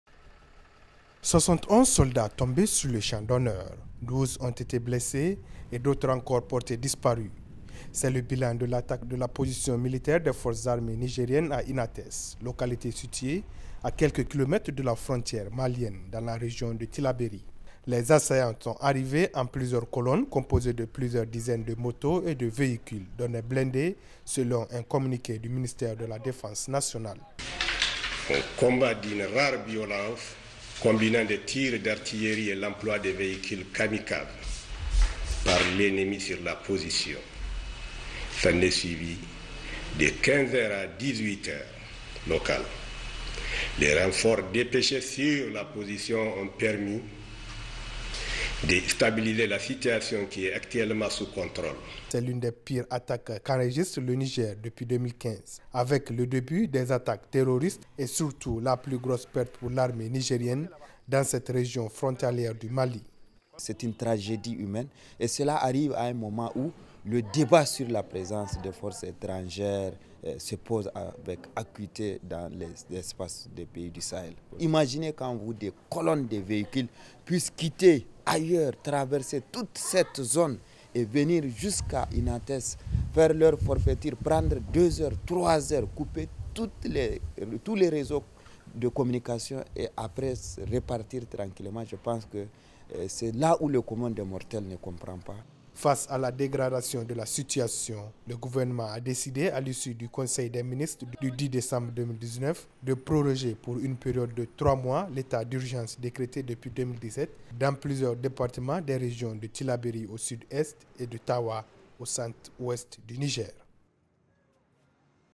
Niger, ATTAQUE MEURTRIÈRE DU CAMP D'INATES
Politique : Attaque djihadiste d'Inates : 71 militaires tués